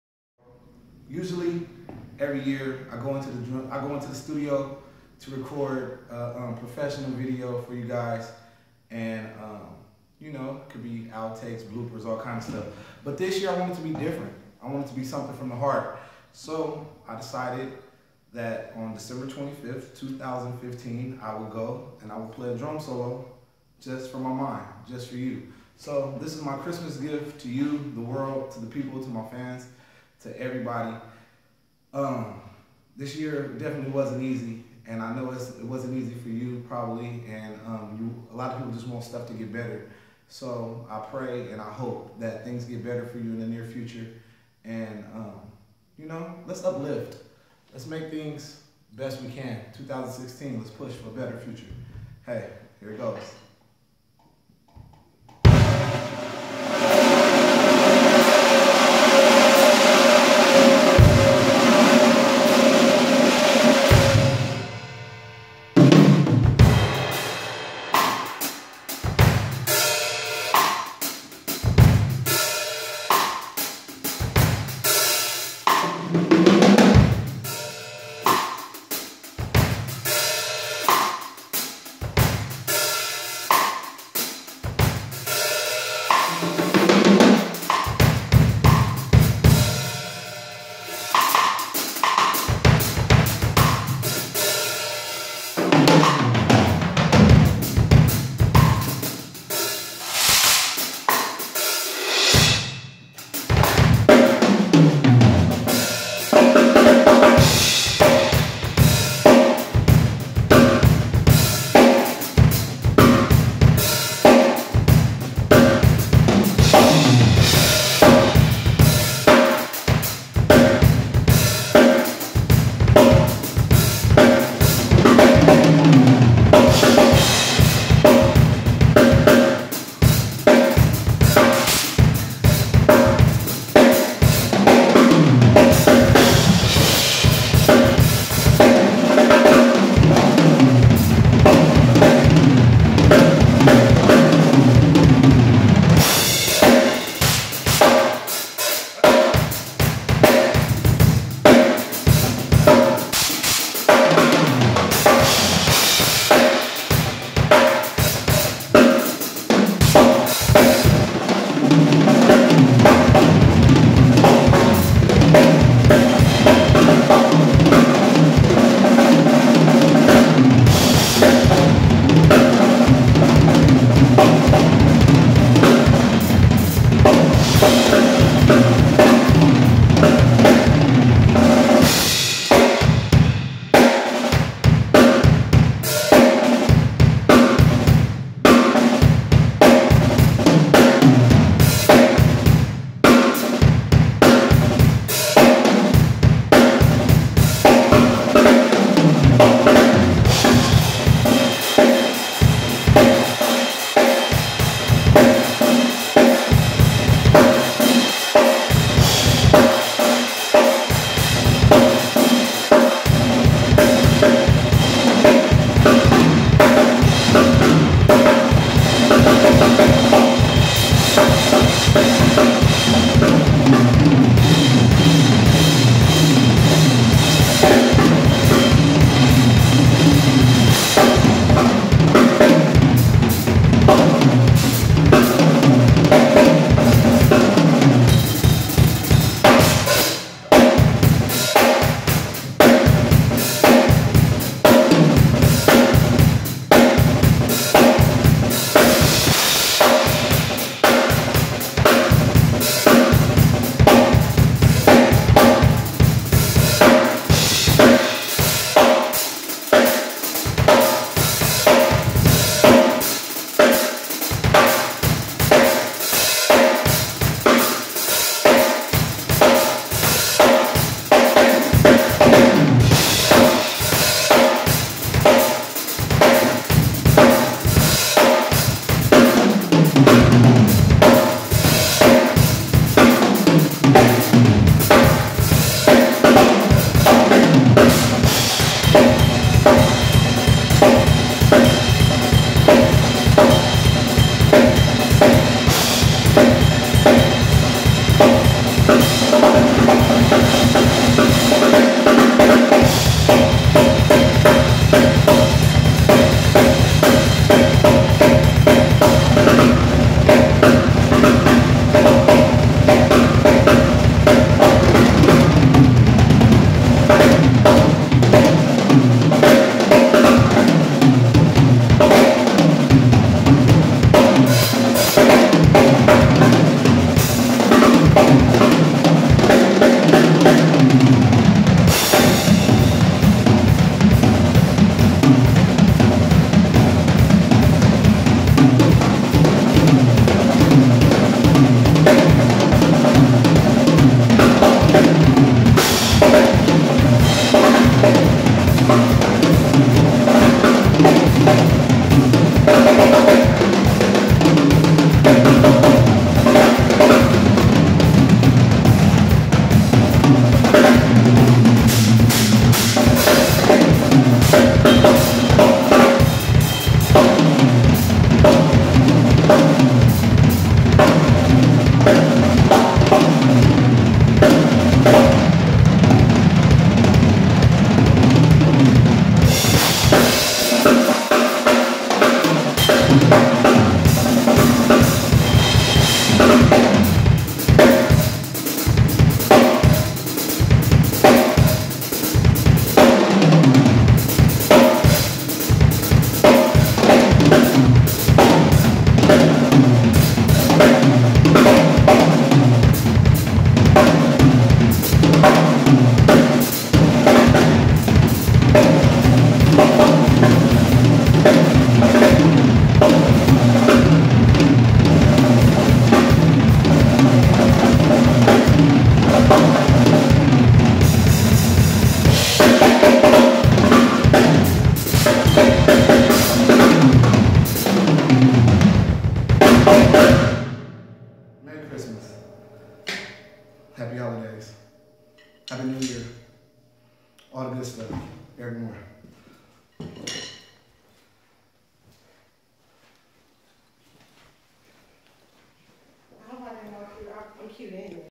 Merry Christmas from Eric Moore
Happy holidays to all. Continue to practice and push your self and great things should come.